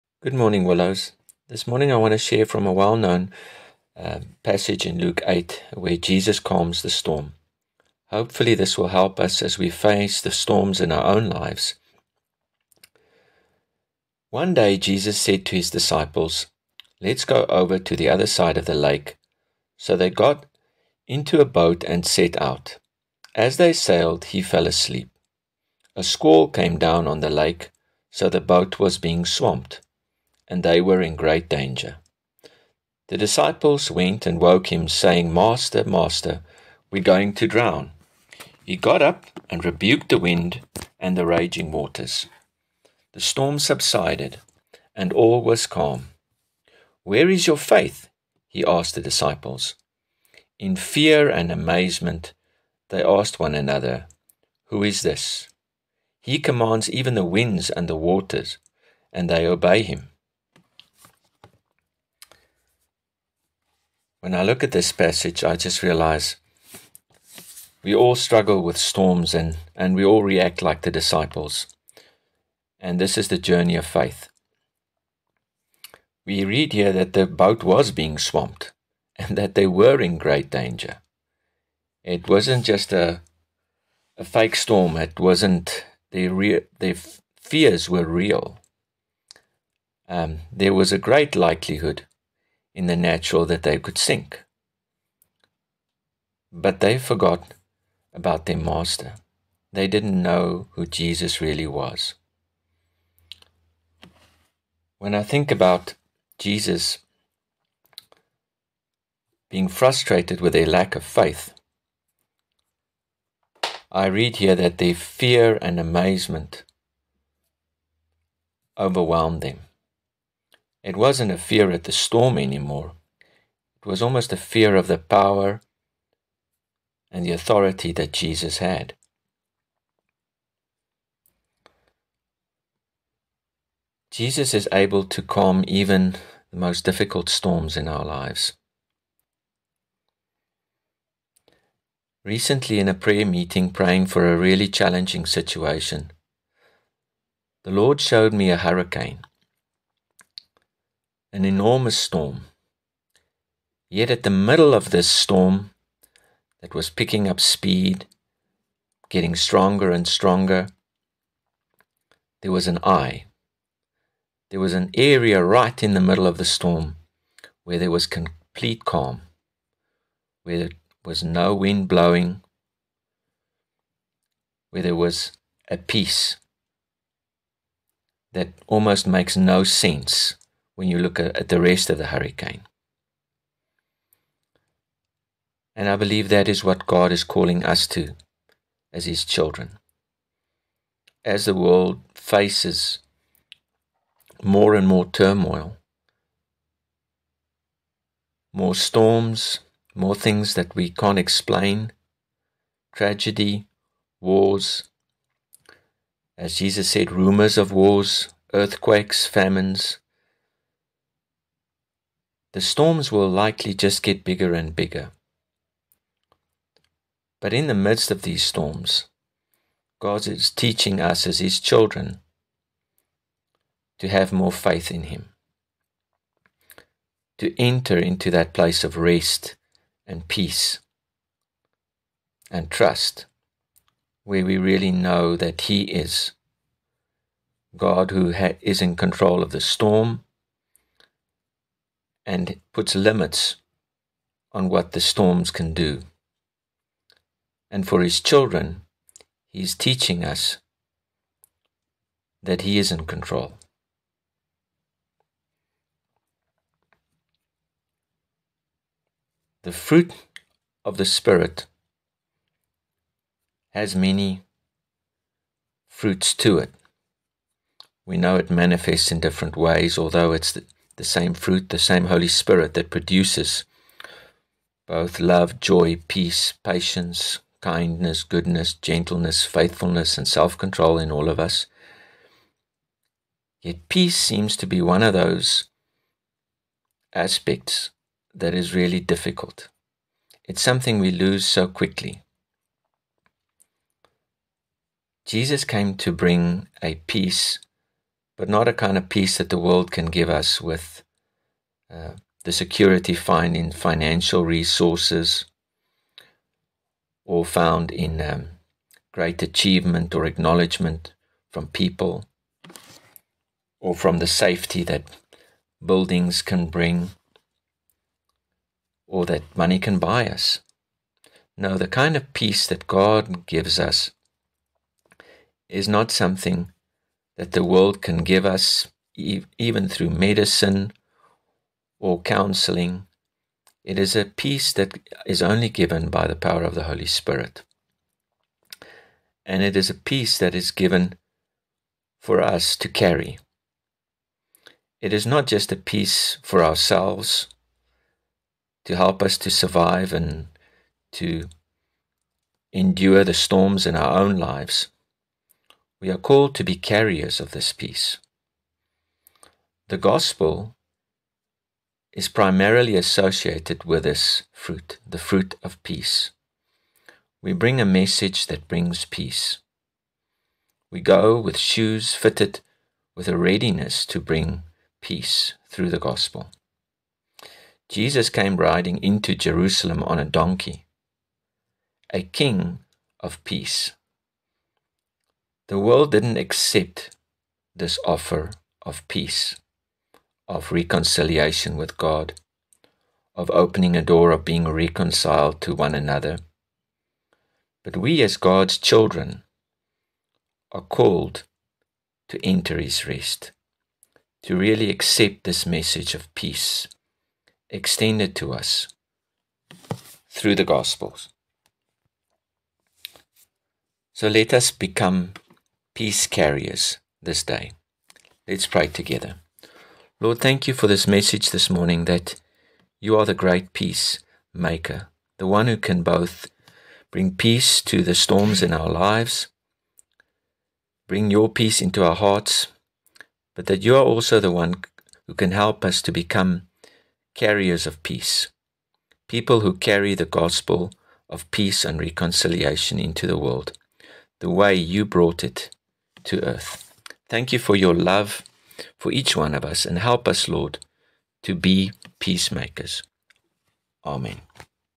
Willows Devotion - Jesus brings calm and peace
Jesus brings peace to the storms in our lives and makes us carriers of peace.

For additional information, please visit our